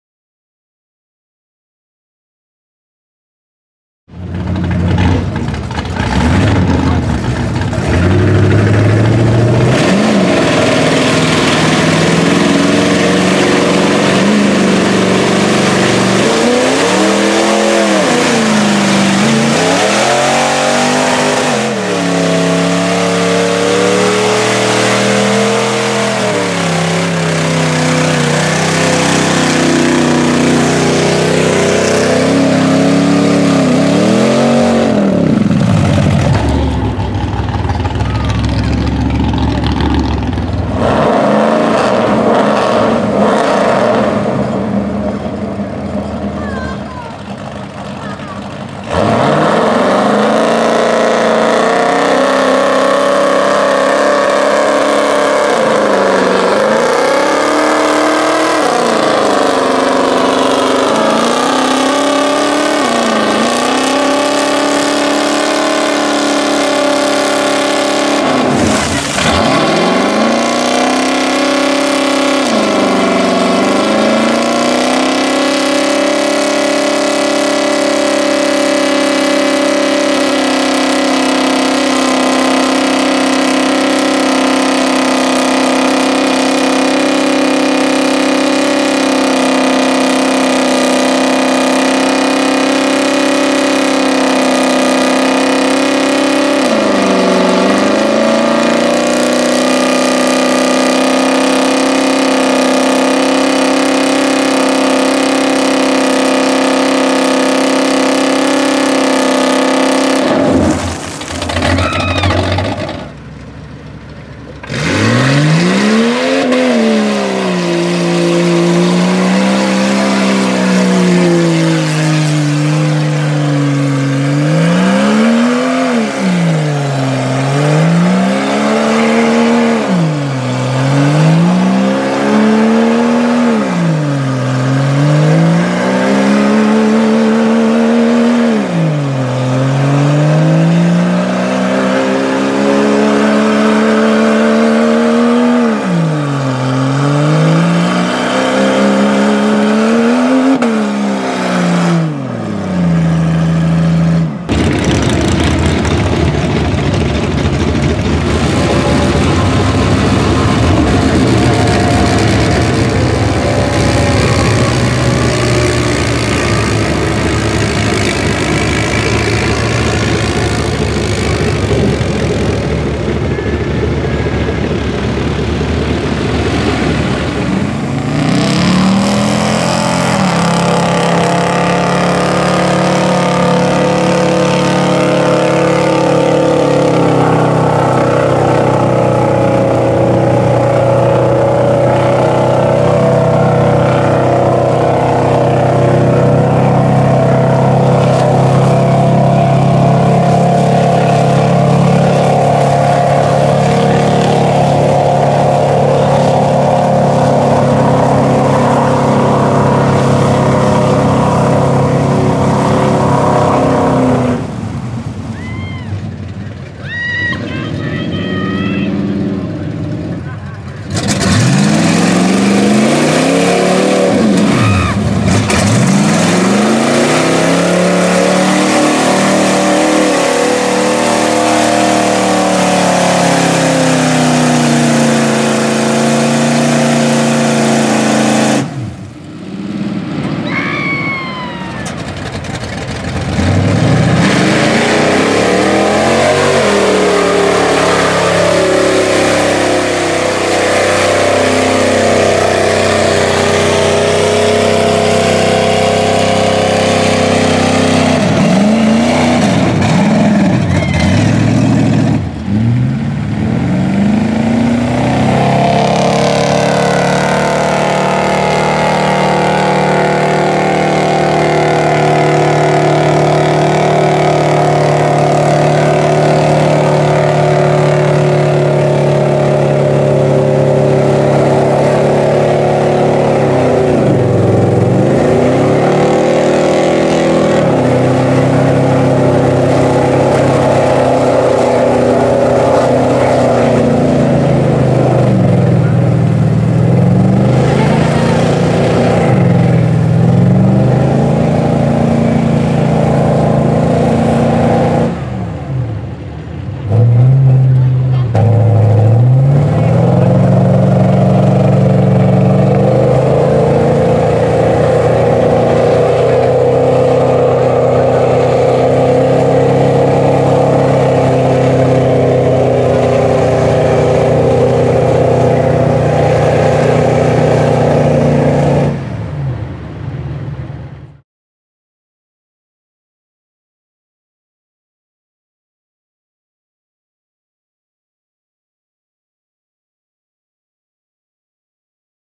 KIDS DRIVING MUD TRUCKS MUD BOGGING!!!
KIDS DRIVING MUD TRUCKS MUD BOGGING!!!  Sak Deep Mud boggers mudding New Years Weekend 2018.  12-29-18
Dads teaching there children how to go mudding!